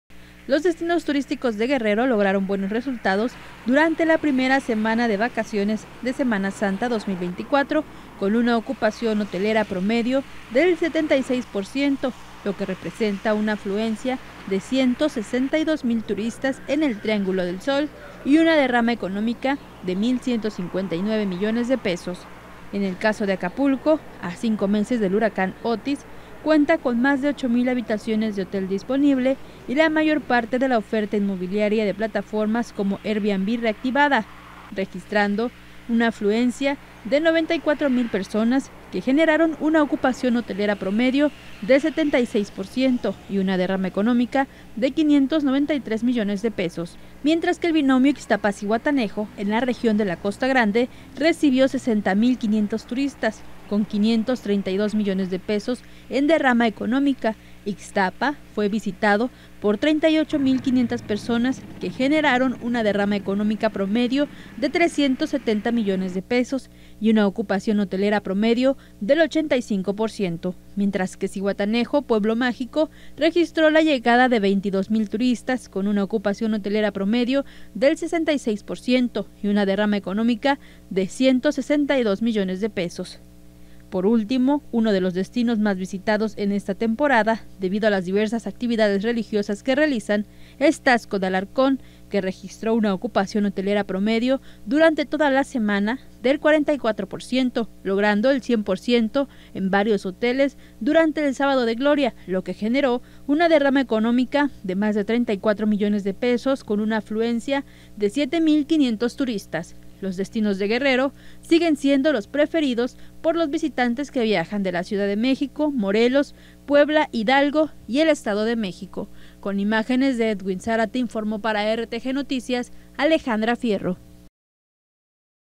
RTGnoticias | En primera semana de vacaciones se registró una derrama económica de 1,159 mdp
Los destinos turísticos de Guerrero lograron buenos resultados durante la primera semana de las vacaciones de Semana Santa 2024, con una ocupación hotelera promedio del 76 por ciento, lo que representa una afluencia de 162 mil turistas en el triangulo del sol y una derrama económica de mil 159 millones de pesos.